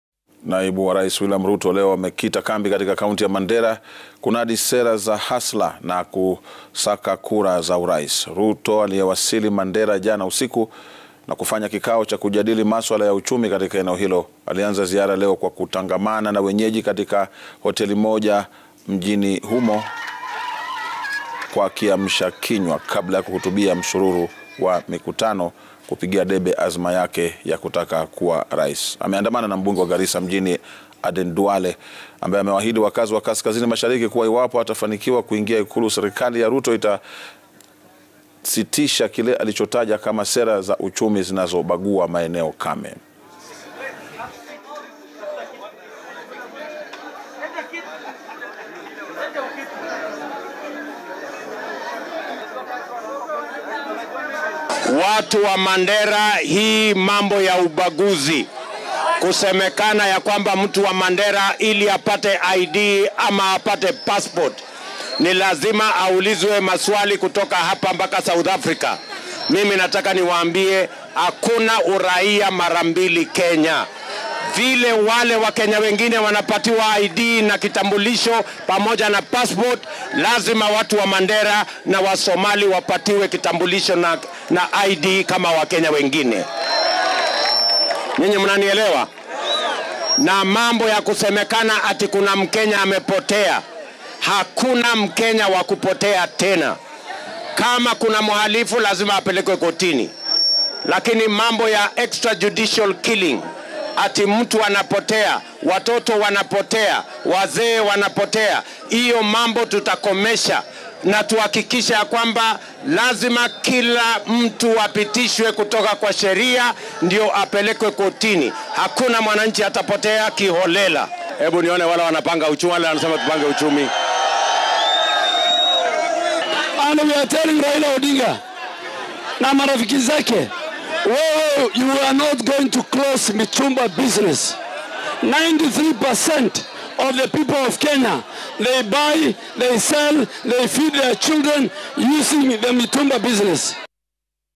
Naibu rais William Ruto aongoza kampeni kaunti ya Mandera
Ruto ameendelea kupigia debe mfumo wa uchumi na biashara
Ameendelea kujipigia debe kuwa bora kwa uchaguzi wa Agosti